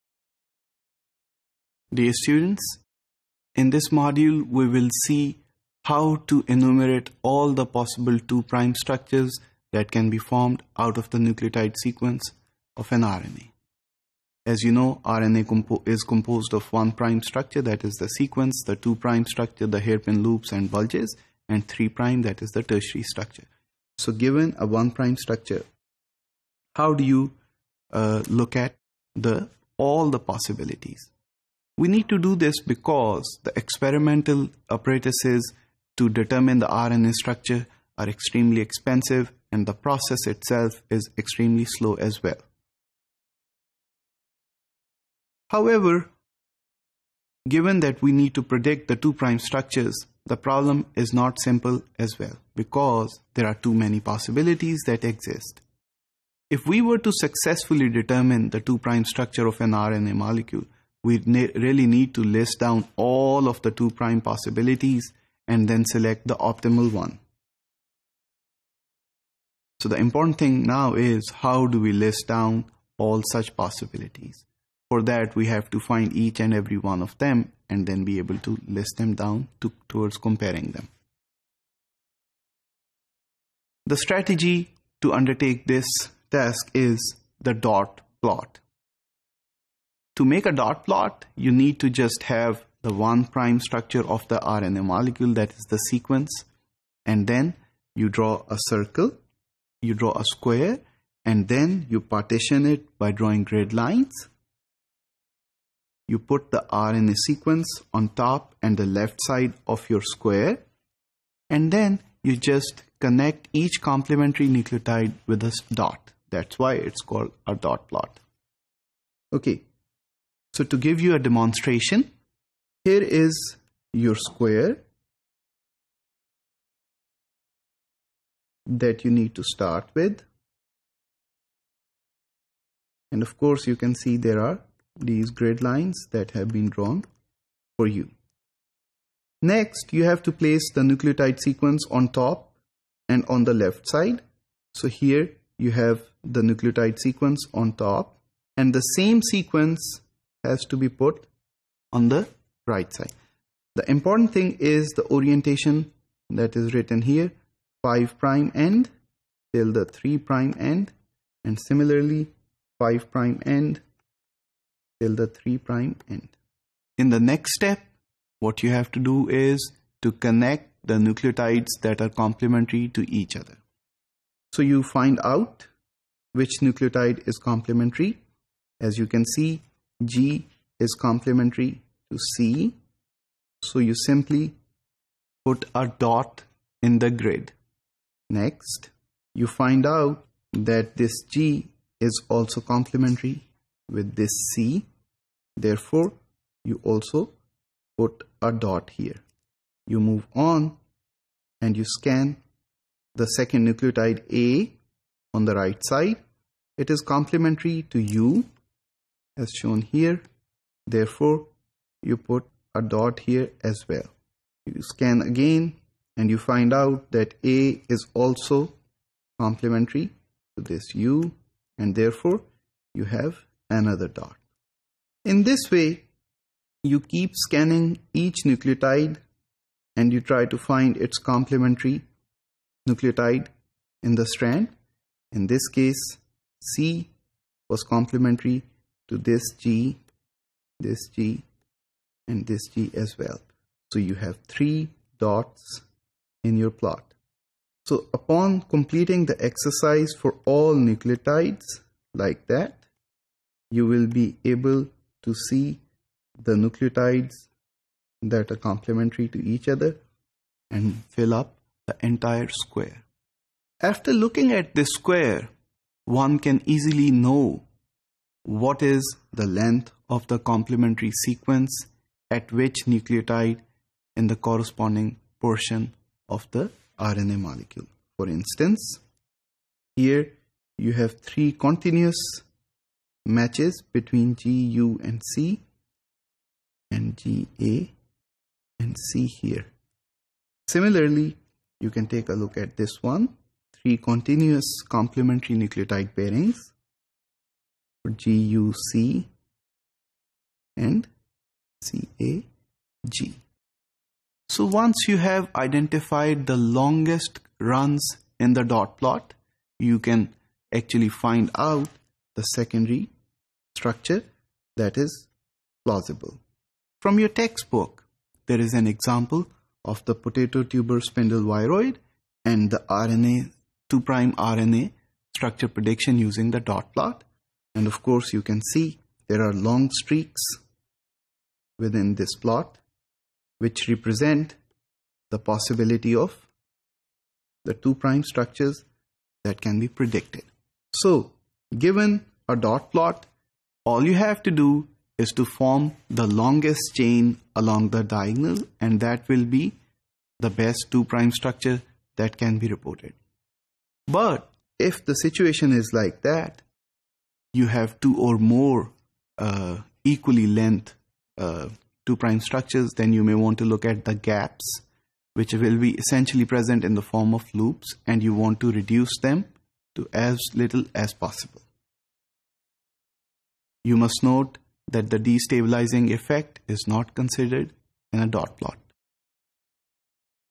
Topic 99 (BIF401 - Bioinformatics I)
Course Code: BIF401
Course Name: Bioinformatics I
Instructor: Dr. Safee Ullah
Topic: Dot Plots for RNA 2' Structure Prediction